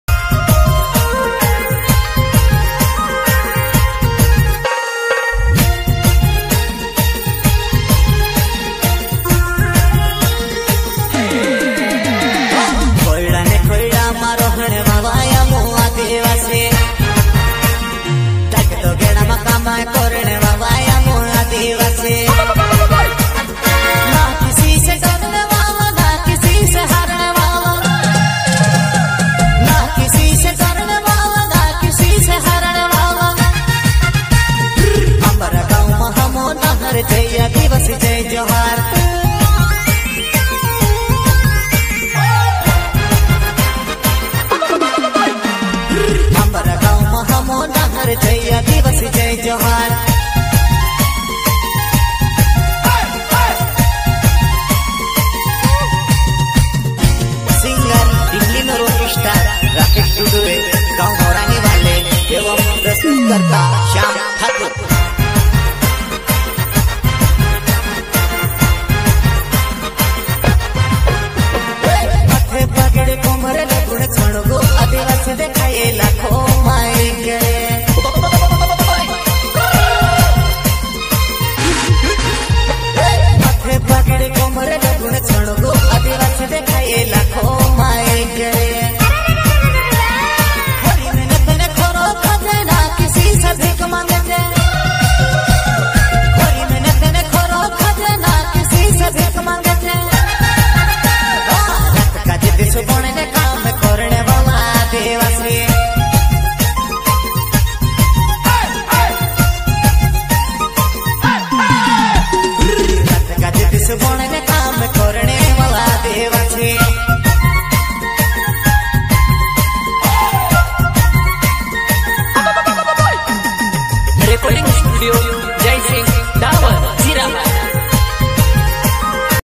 ना किसी से डरने वाला 💪 ना किसी से हारने वाला ओगणा क्षेत्र में आदिवासी का डांस जबरदस्त आदिवासी भाइयों
मेरे वीडियो डांस एक्टिंग वीडियो है दोस्तों आगे फुल सपोर्ट करेंओगणा अपना क्षेत्र में पहली बार डांस अपना आदिवासी ओगणा वीडियो आपको अच्छा लगे लाइक शेयर सब्सक्राइब आगे सपोर्ट करें सभी दोस्तों को जोहार कैलाश जय जोहार चैनल को सब्सक्राइब करें